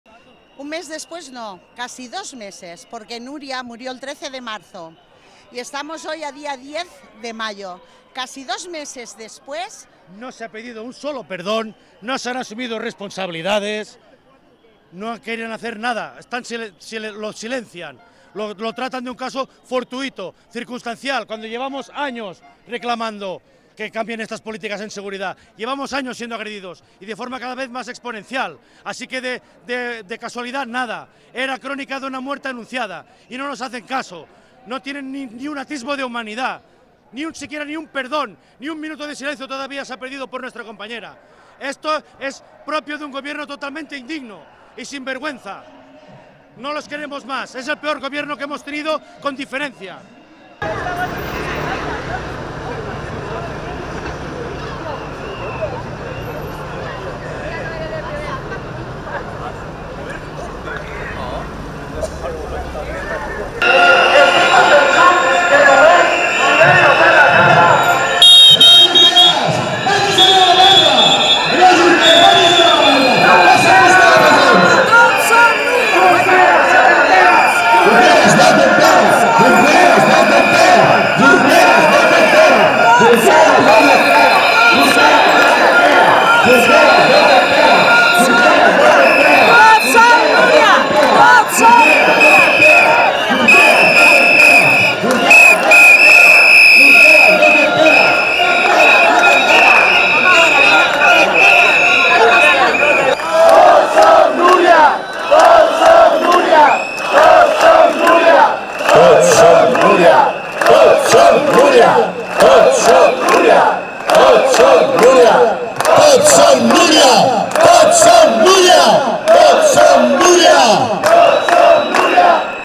400 personas en la protesta de funcionarios prisiones en Barcelona piden "volver vivos a casa"
Cerca de 400 personas han acudido este viernes a la manifestación de funcionarios de prisiones y los sindicatos Csif, Acaip y Marea Blava para protestar por la muerte de Núria, la cocinera de la cárcel de Mas d'Enric, en Tarragona, a manos de un preso, y pedir mejoras de seguridad: "Queremos volver vivos a casa".